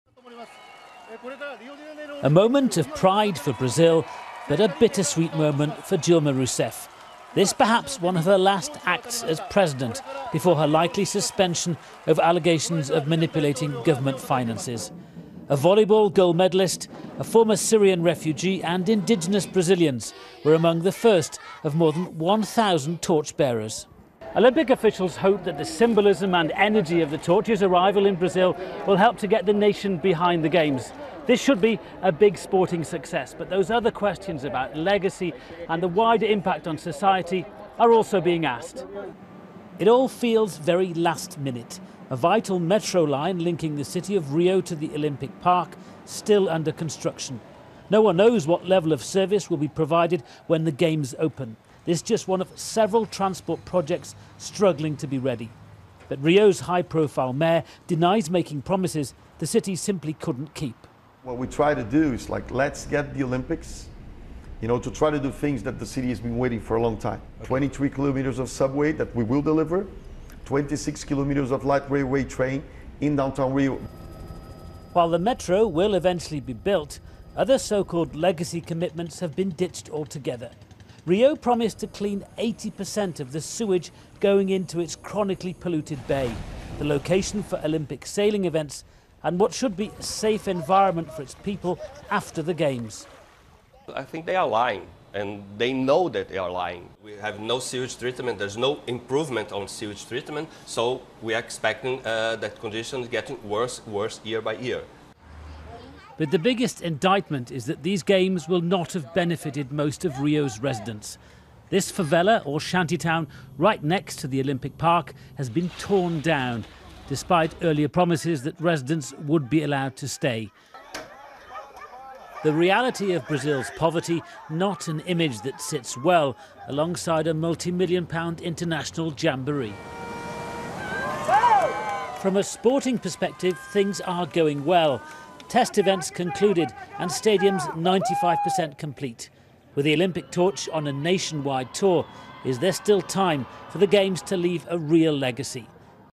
Olympic flame arrives in Brazil
The Olympic torch has arrived in Brazil.
It was flown inside a small lantern on a special flight from the Swiss city of Geneva to Brasilia.
The first torch bearer was volleyball player Fabiana Claudino.